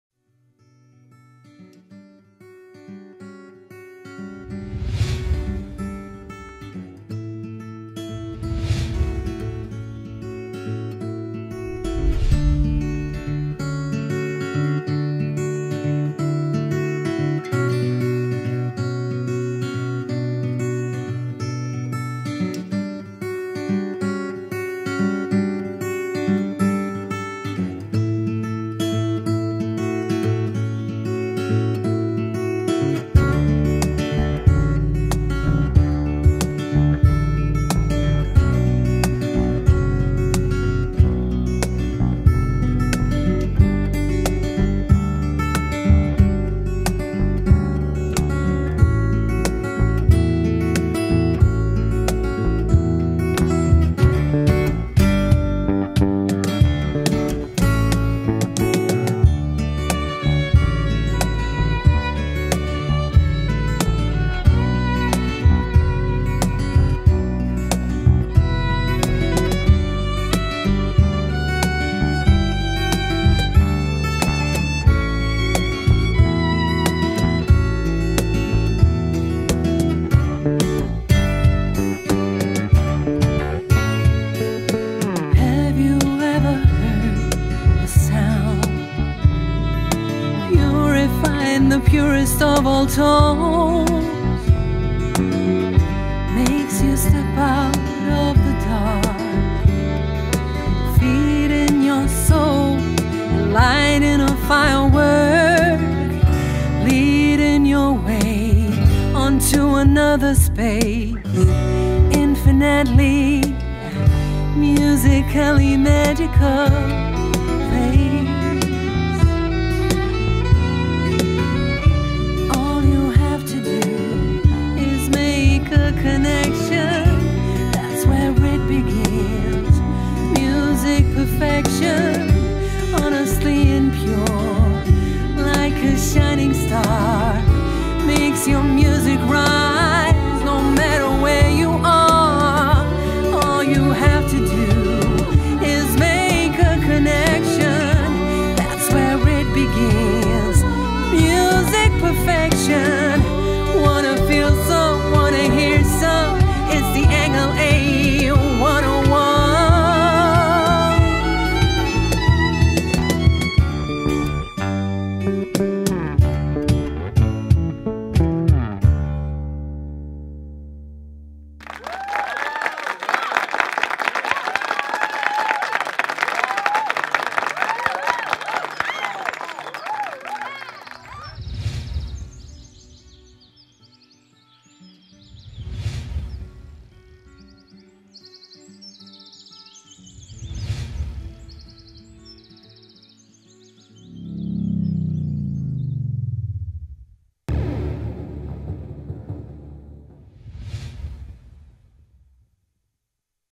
ENGL TV - Amazing A101 Acoustic amp song!
The ENGL A101 Acoustic amp song!

The ENGL A101 is a 150 W all-round acoustic amplifier, designed to fulfill the needs of every performing artist. This amp is not only for acoustic guitars, but also for vocals and all other acoustic instruments. It is even possible to connect instruments with higher outputs to it like keyboards. This makes it the perfect tool for smaller gigs and even makes rehearsing wherever you are a totally new and impressive experience.

Meet at your friend’s house, connect one or two guitars (Channels 1-3), a mic (Channel 1-2) or maybe a keyboard (Channel 3) to the A101 and start practicing with a sound that gives you the feeling of being live on stage. Invite some friends and play a livingroom gig with the ENGL A101. You need nothing else.

Even when you play at a bigger venue you can easily use the Rec Out and connect it to a second A101 so that everyone will be able to hear you loud enough. Channel 4 of the A101 gives you the possibility to keep the party going on all night long after your gig by simply connecting e.g. a MP3-Player to it. You can also use this Channel to play along to your favorite songs. Either you just want to jam to them or practice, it is a lot of fun.

With its warm and rich midrange and smooth and silken treble, the A101 delivers an organic sound. It is equipped with four different effects, which top off the many possibilities this amp offers.

The natural and sophisticated design of the A101 focuses on an extensive appeal and due to the real branding on the sites, every amplifier is as unique as you are, the artist who is using it!